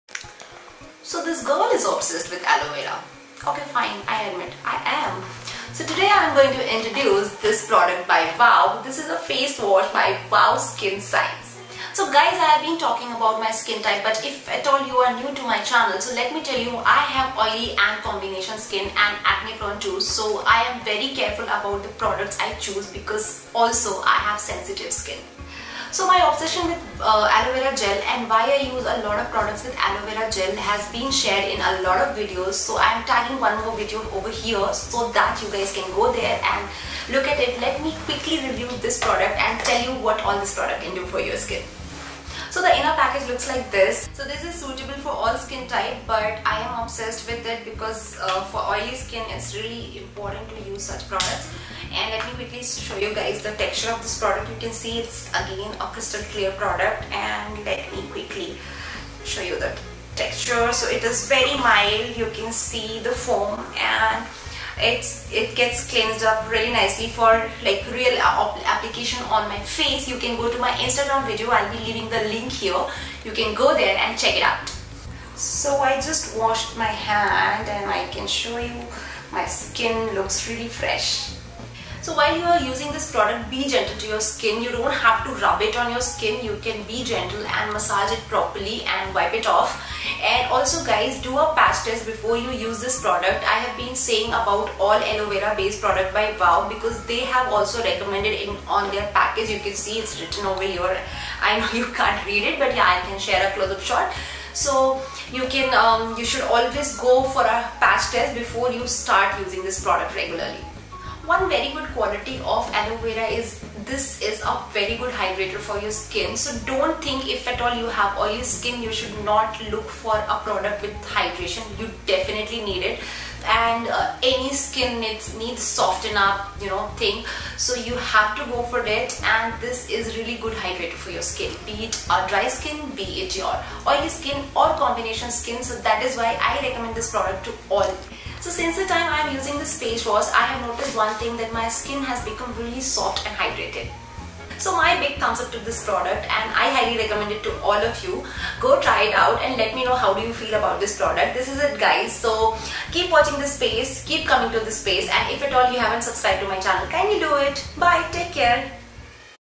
WOW Skin Science Aloe Vera Gel face wash For Skin Hydration
Hey guy, I think I need not tell me obsession towards aloe vera gel-based products. I am in love with this wow skin science aloe vera gel face wash. I have been using all aloe vera gel-based products from this brand. I just love their wow skin science aloe vera gel. But sometimes it's difficult to take out time for DIYs and then I decided to try a face wash itself and make my morning skincare routine even simpler.